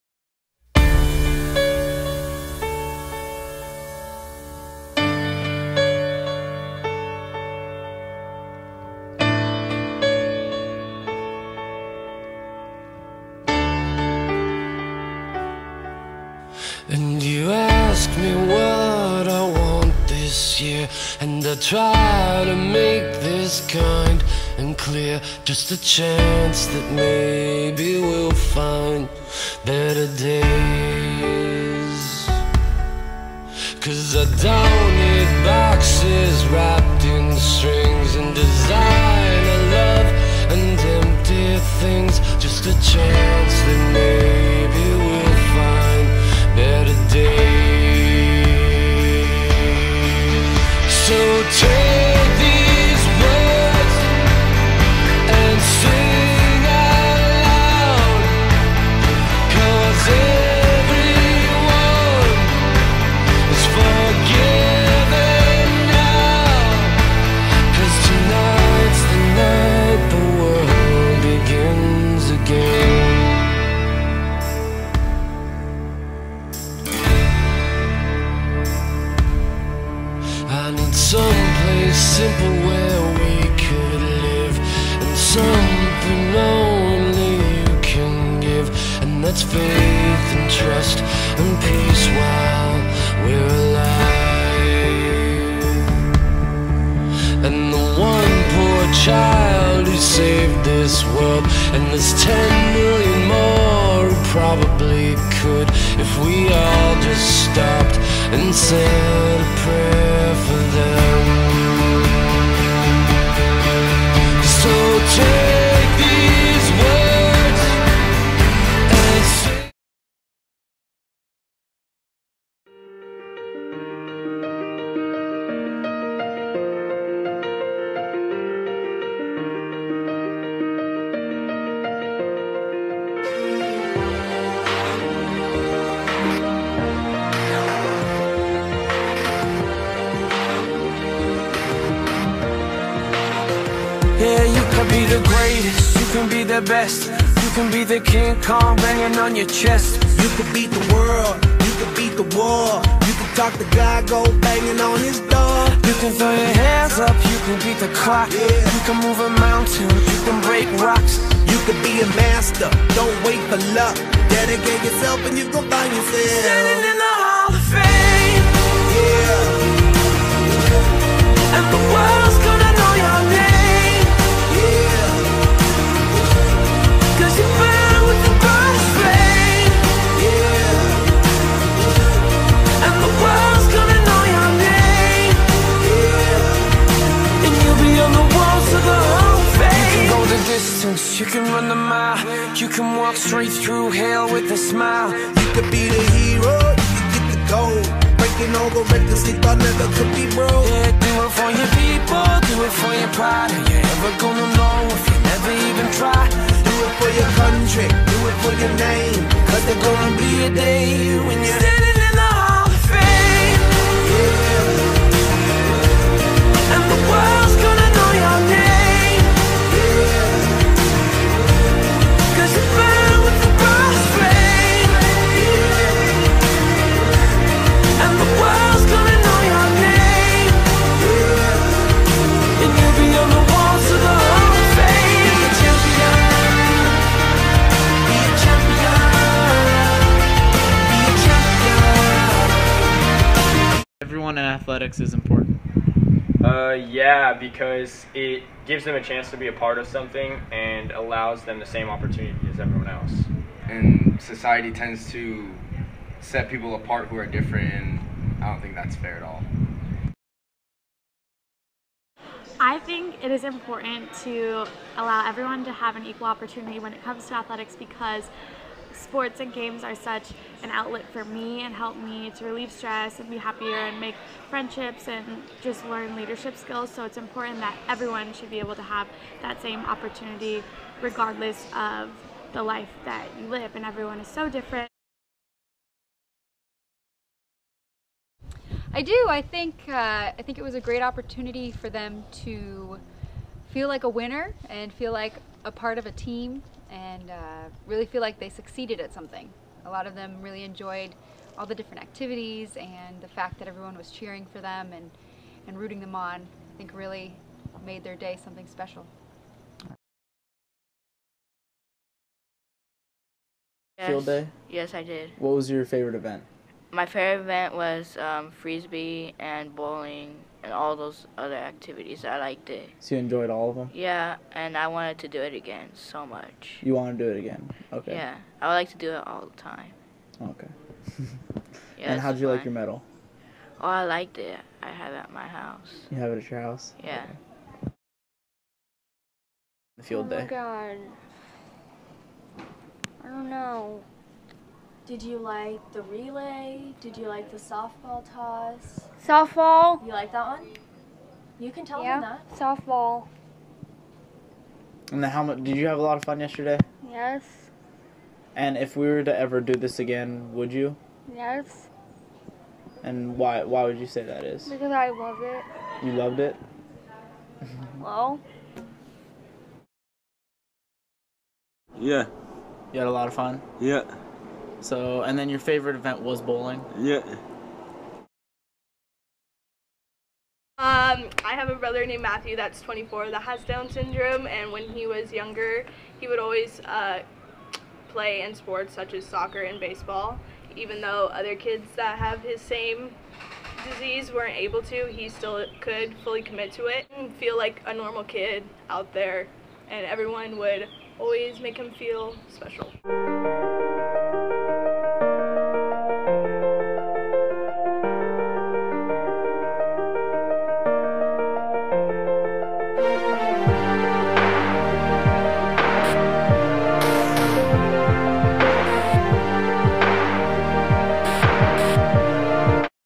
Change the World: Sports Edition 2015
Steele Canyon High School students put on a field day for these young boys and girls. This was our Change The World project. Enjoy.